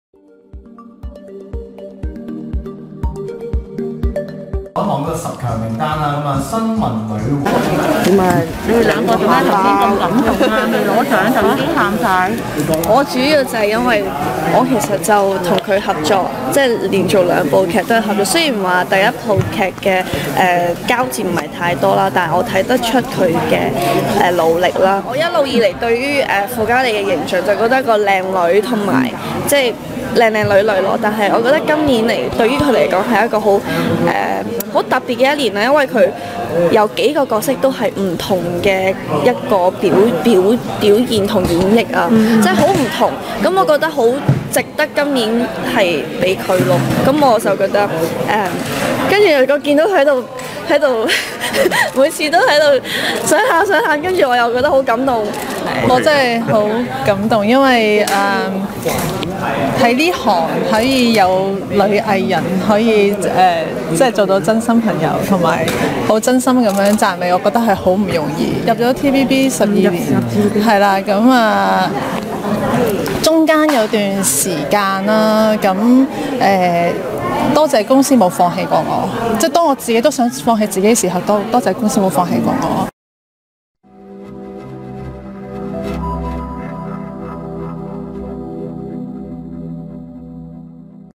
傅嘉莉陳星妤入圍10強感觸爆喊 「飛躍進步男藝員」10個變11個
《萬千星輝頒獎典禮2023》將於明年1月14日移師澳門舉行，今日在電視城公布16個獎項10強名單，新加入了大灣區4個獎項，分別「大灣區最喜愛TVB劇集」「大灣區最喜愛TVB男女主角」及「大灣區最喜愛TVB綜藝及資訊節目」，而「飛躍進步男藝員」因同票關係故有11位。